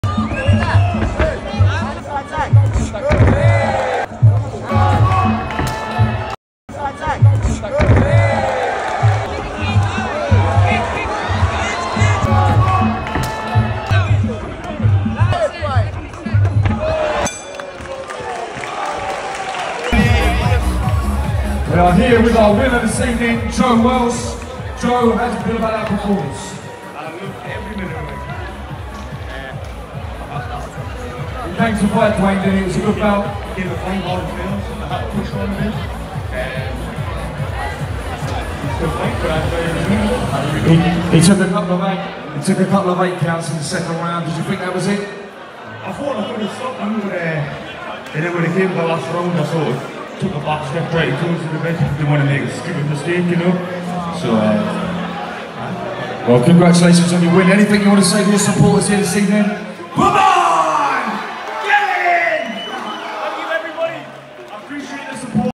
Joe Welch
As Joe prepares himself for a chance at the WBC European Title next month on VICTORY12 , we take a look back at his highlights from VICTORY11